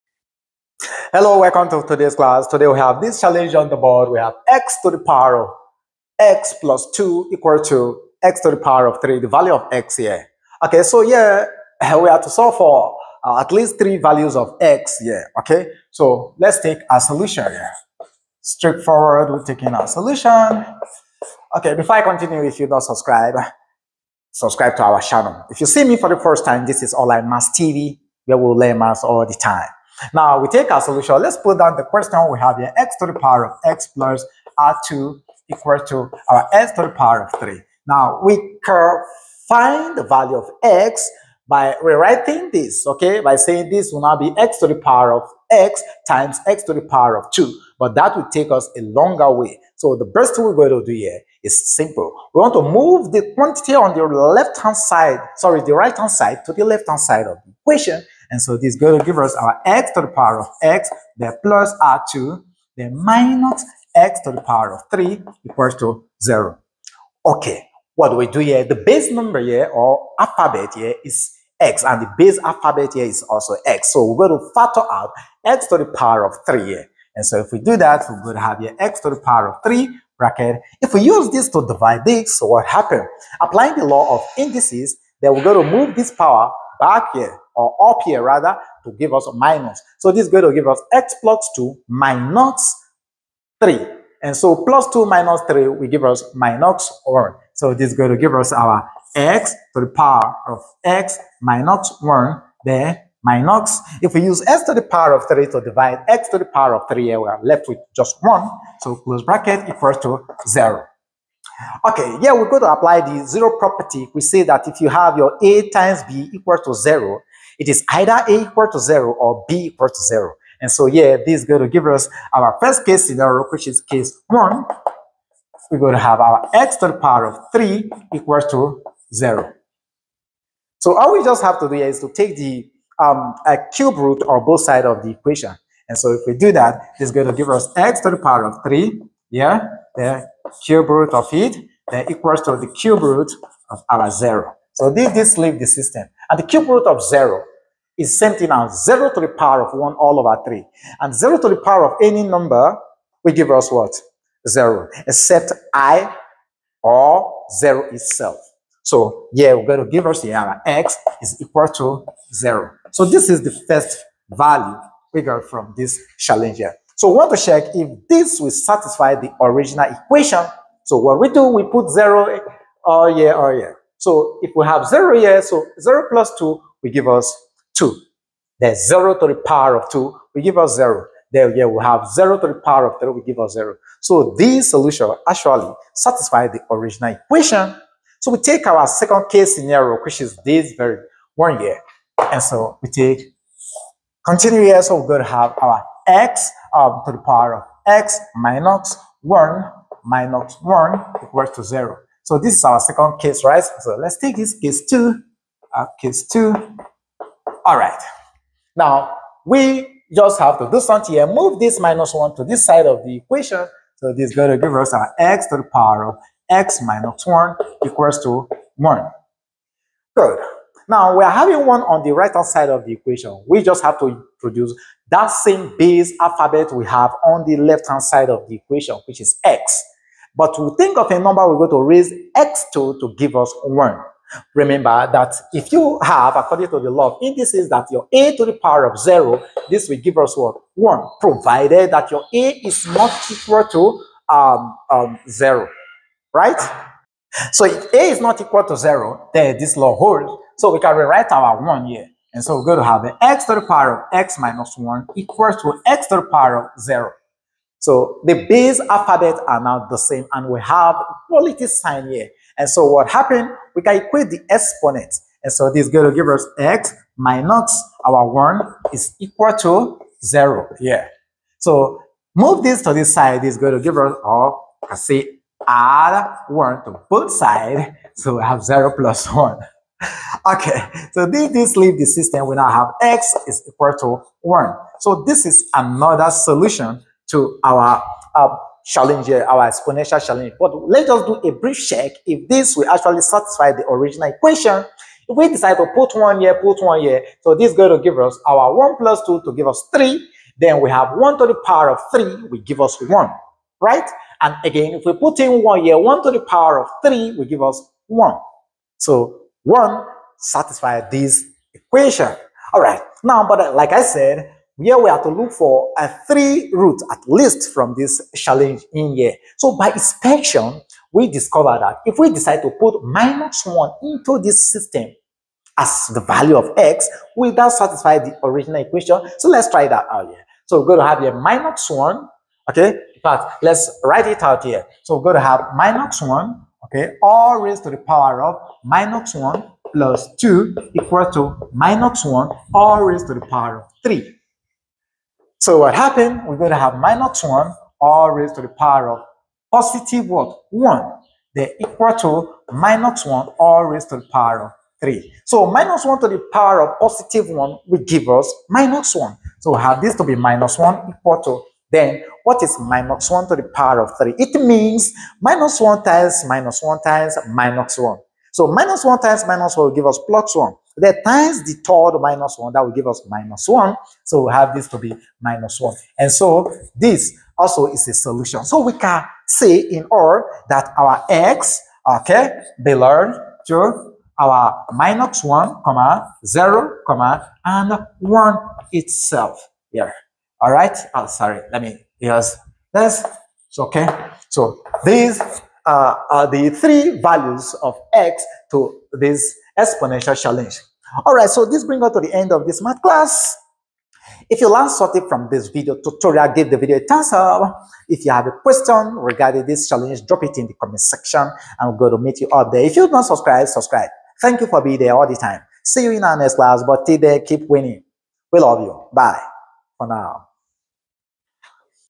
A Nice Exponential Equation | Can You Solve This Math?
I will show you how to solve this equation neatly.
Just watch from the beginning to the end.